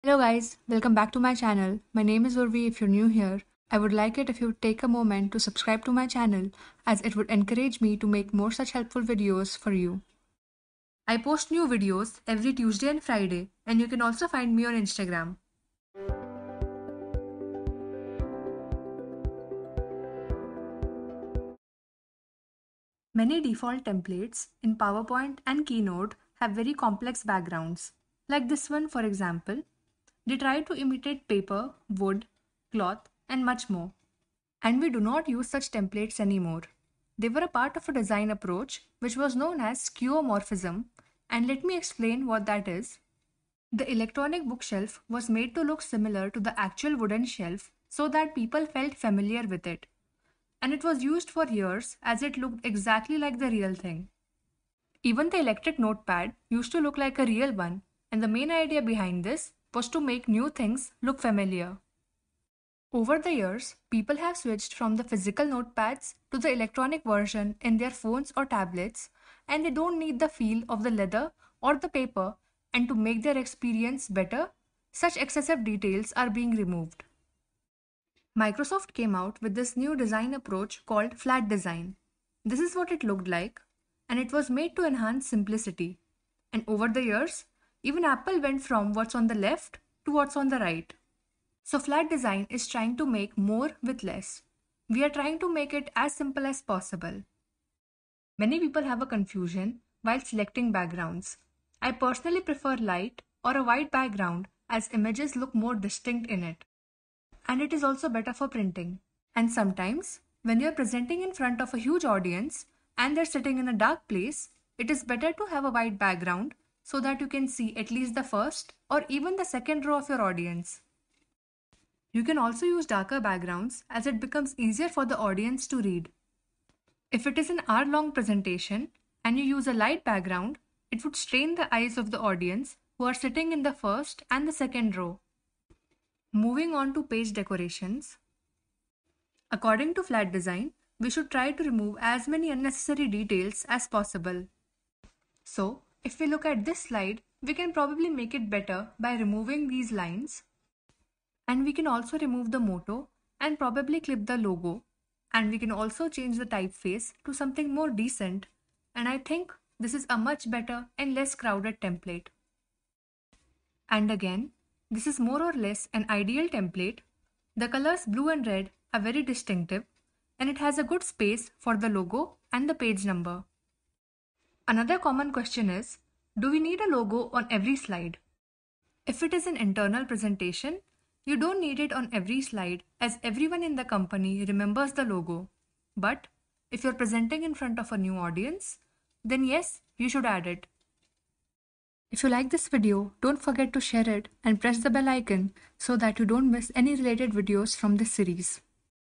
PowerPoint Presentation Skills Full Course: (5) Background & Page decoration
In this video you will learn about the different backgrounds offered in Powerpoint, whether to have a light or a dark background, a concept called skeuomorphism, and why most of the presentations now use a flat design approach.